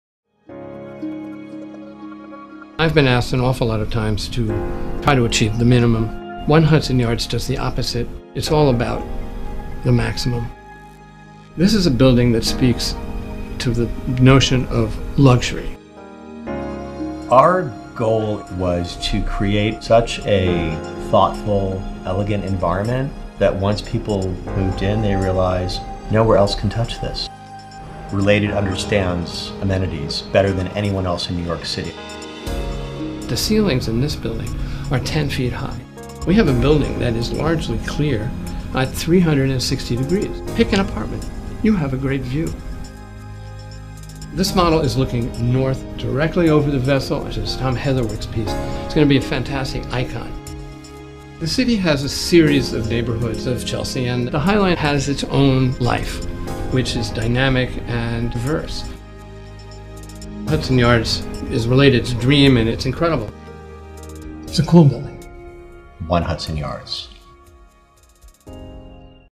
One Hudson Yards
The 33-story building features bold architecture by Davis Brody Bond, with layouts by Executive Architect Ismael Leyva Architects, and rich interiors by the award-winning Andre Kikoski Architect that were custom designed to meet the needs of sophisticated and discerning New Yorkers.

One Hudson Yards is situated at 530 West 30th Street along the High Line and at the southern entrance to Hudson Yards. The sleek stone and glass building comprises 178 apartments. Each apartment is generously laid out with 10-foot ceilings, wide-plank grey oak floors, and extra-tall windows in a modern curtain wall façade that reveal breathtaking views of Hudson Yards, the Hudson River, West Chelsea and Downtown Manhattan.

Focused on integrating art and architecture throughout the building, every piece in the building’s art collection is about process, production, and materiality. The building is surrounded by and filled with art- it overlooks Heatherwick Studio’s interactive design piece Vessel and The Shed, New York’s first arts center to commission new work across the performing arts, visual arts, and popular culture. Source by Related Rentals.